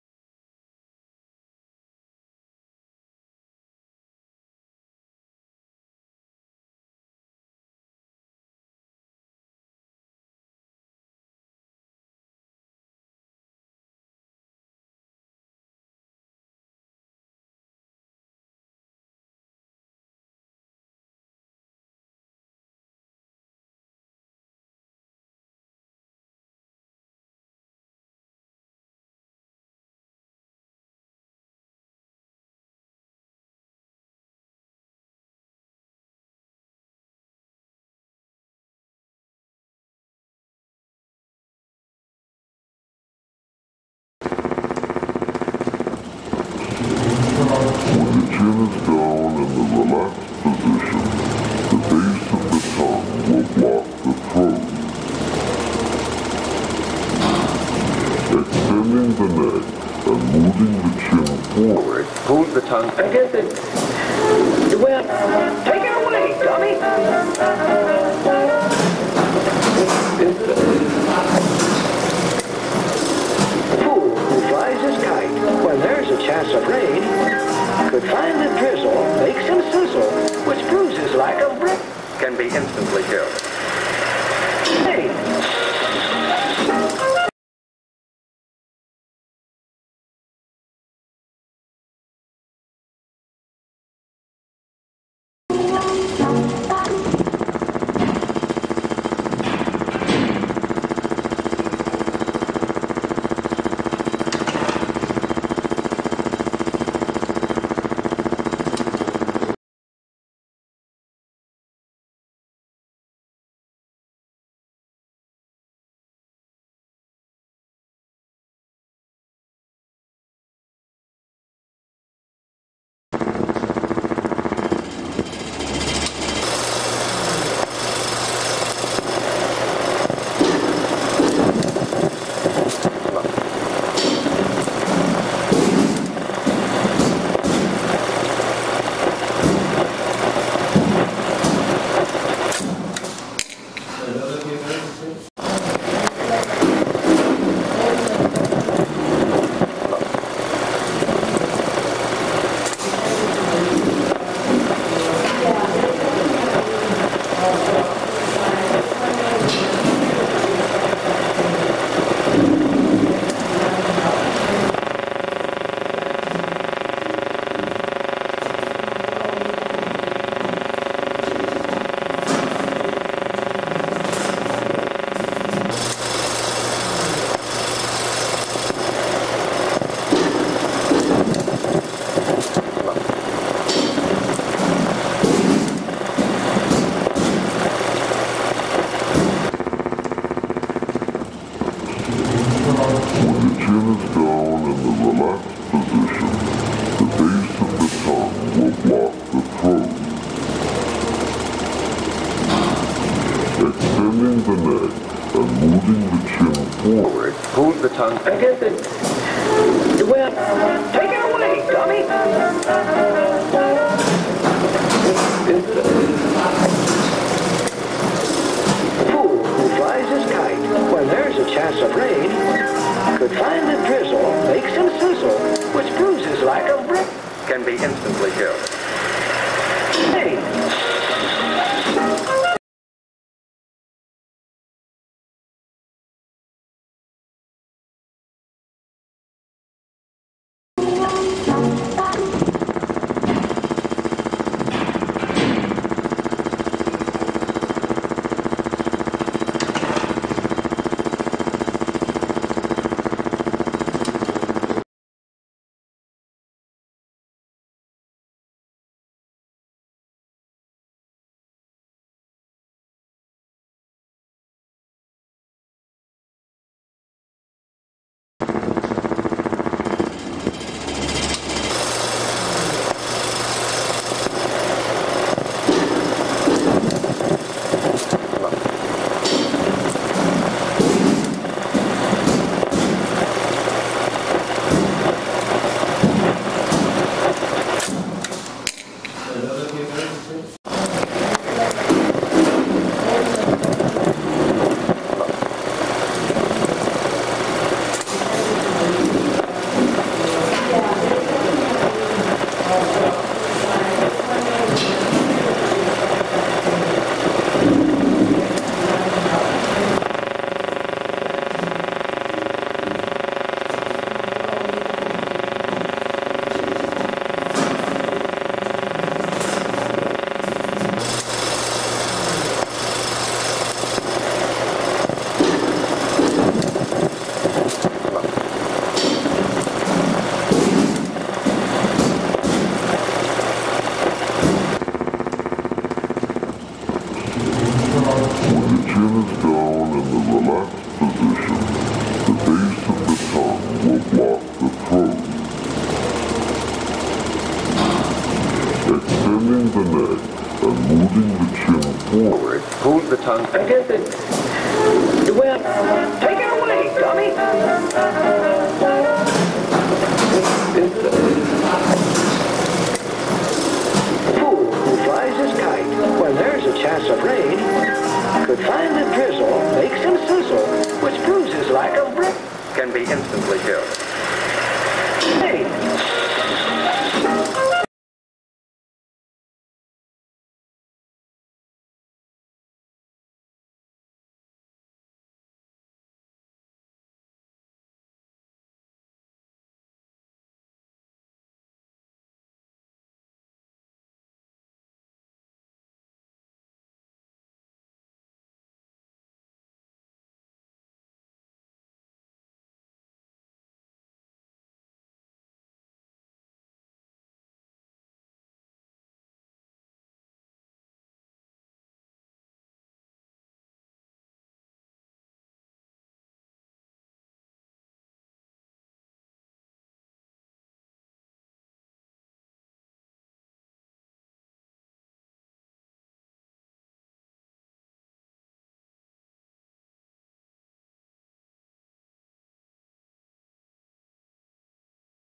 A Chance Discourse of Mouthsters
16mm project - Chance Discourse, A Conversation of Talkative Monsters.

Films by Chantal Beaulne and Naime Tsademir

Emily Carr University of Art and Design